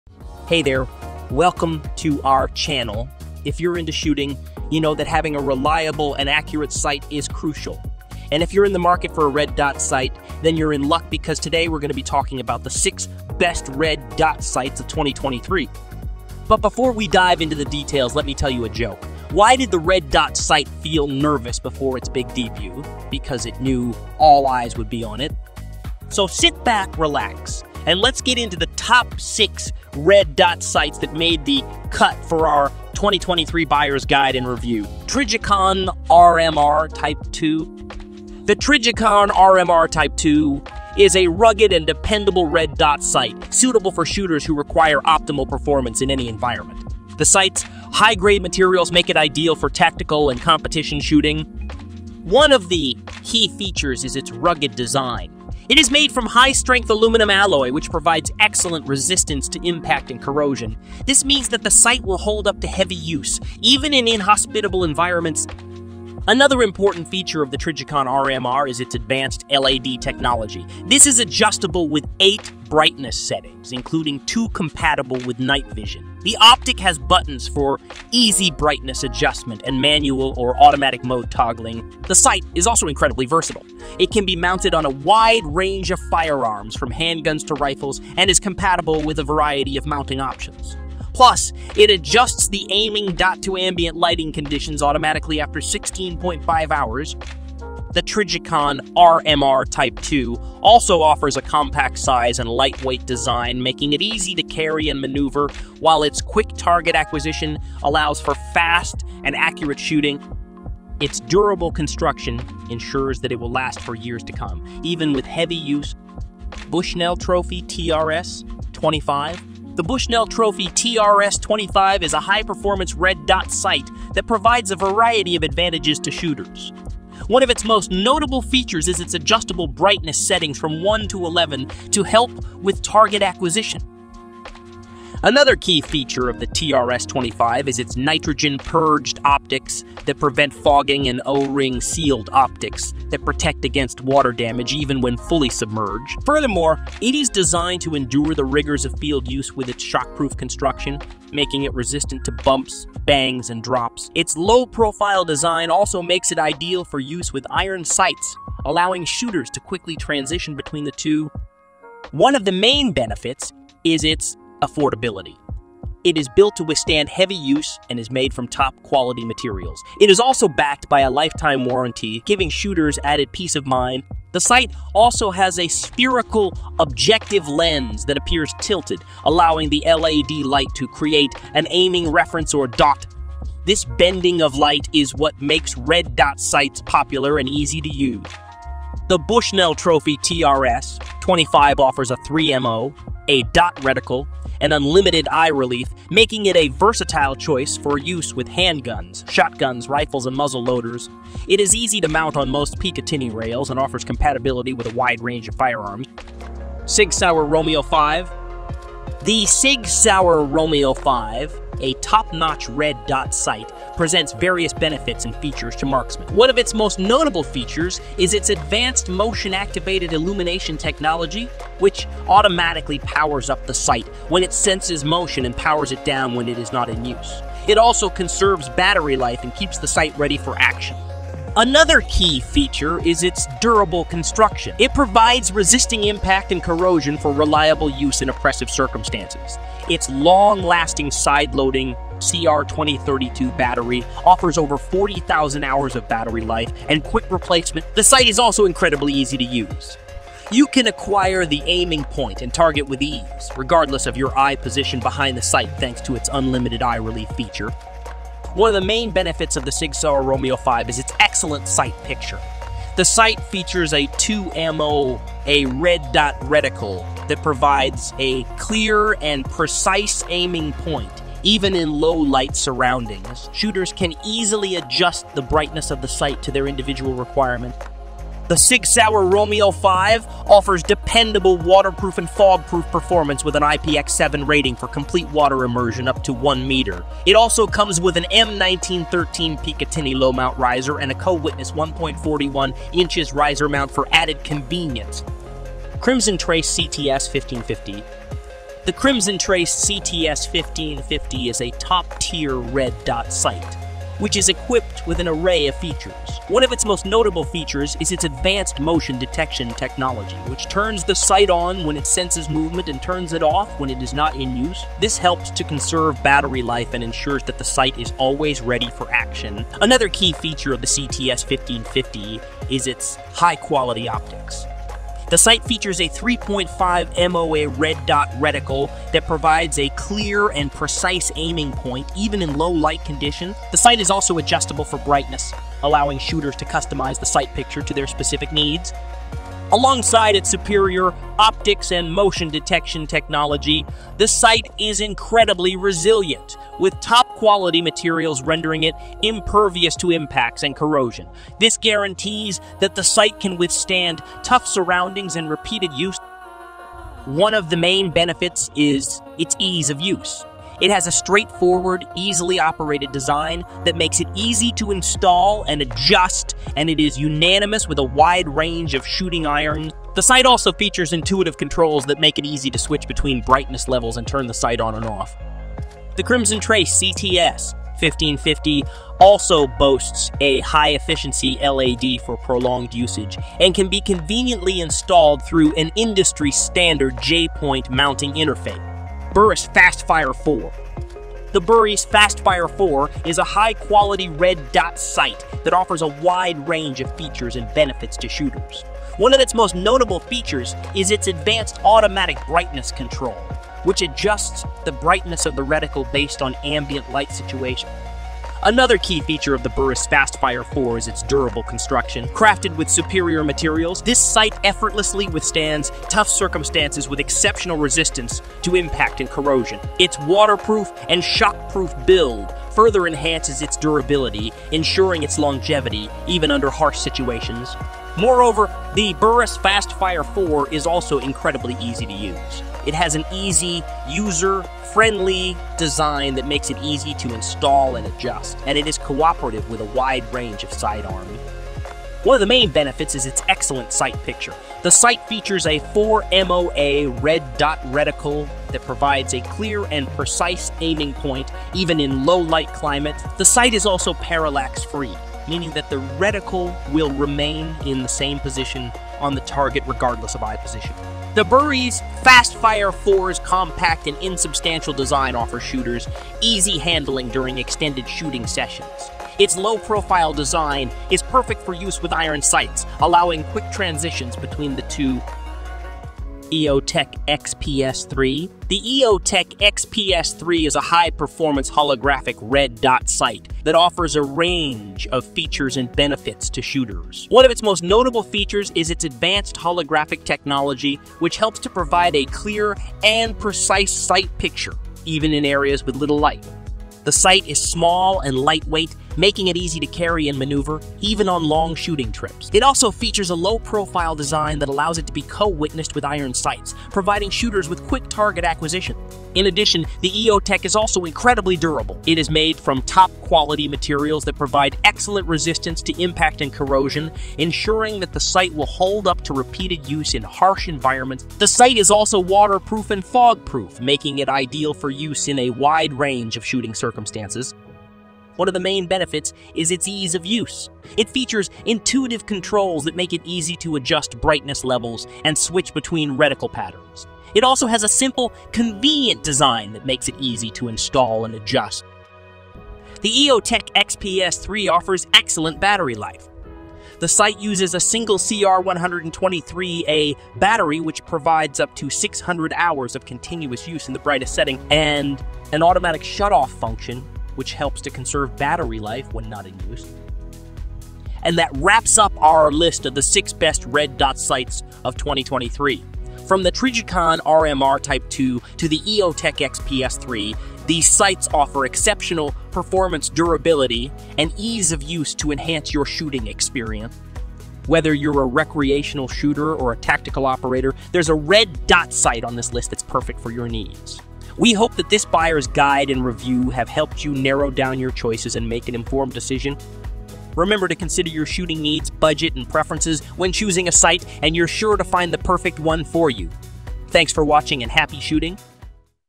The 6 Best Red Dot Sights Of 2023 - Buyers Guide and Reviews
If you're into shooting, you know that having a reliable and accurate sight is crucial. And if you're in the market for a red dot sight, then you're in luck because today we're going to be talking about the 6 best red dot sights of 2023. But before we dive into the details, let me tell you a joke. Why did the red dot sight feel nervous before its big debut? Because it knew all eyes would be on it.  So, sit back, relax, and let's get into the top 6 red dot sights that made the cut for our 2023 buyers guide and review-.

Trijicon R M R Type 2-
Bushnell Trophy T R S-25
Sig Sauer Romeo 5-
Crimson Trace C T S-1550
Burris Fast Fire 4
E O Tech X P S 3-

Whether you're a recreational shooter or a tactical operator, there's a red dot sight on this list that's perfect for your needs. We hope that this buyer’s guide and review have helped you narrow down your choices and make an informed decision.

Remember to consider your shooting needs, budget, and preferences when choosing a sight, and you're sure to find the perfect one for you.